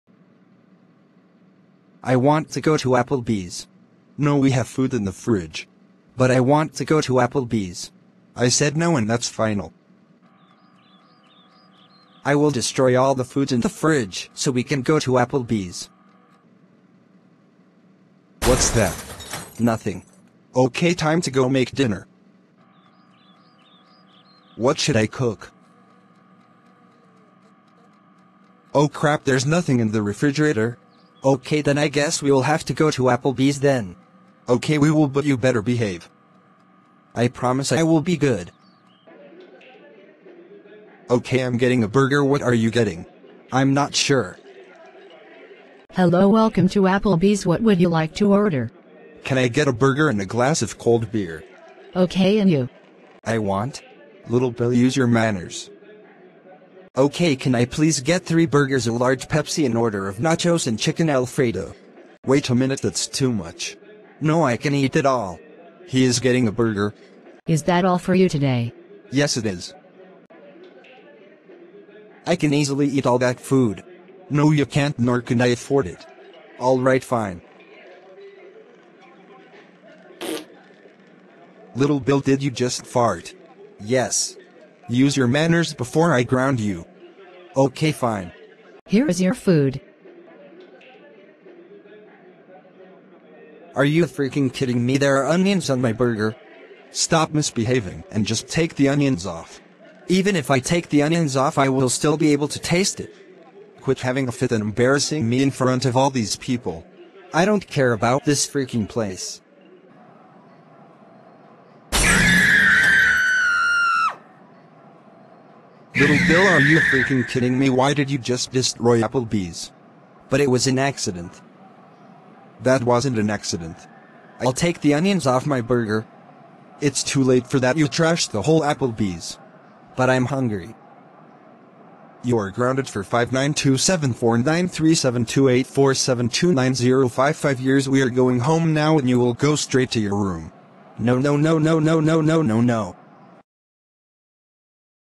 Little Bill gets grounded for misbehaving at Apple Bee's
Little Bill goes to Apple Bee's with his dad because he destroyed all the food in the refrigerator. Then Little Bill goes to Apple Bee's on his brave mission to embarrass his dad. This quest was successful.